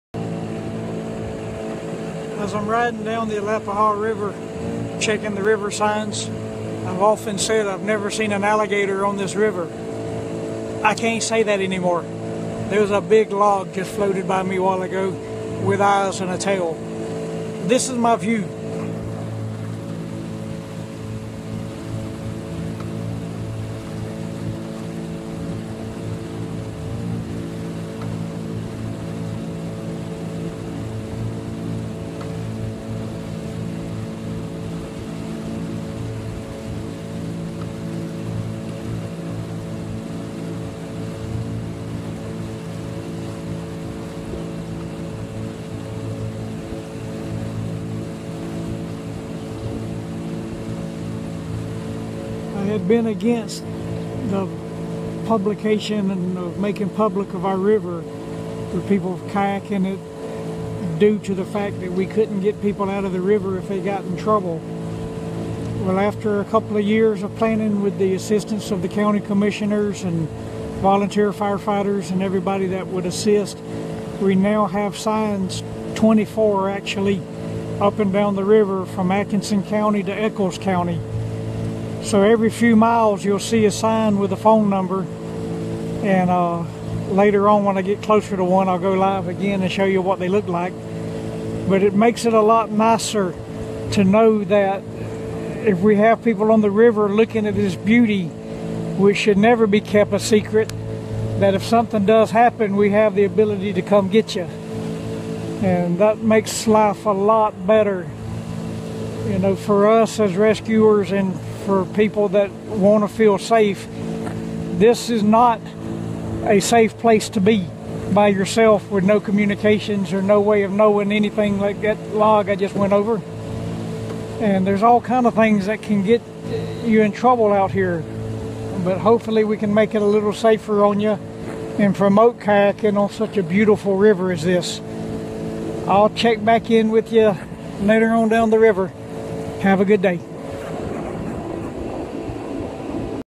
Alapaha River Beauty should not be hidden, Randy Patten 2018-04-07
Live video from the Alapaha River, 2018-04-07
Video by Randy Patten for Lanier County, GA Emergency Management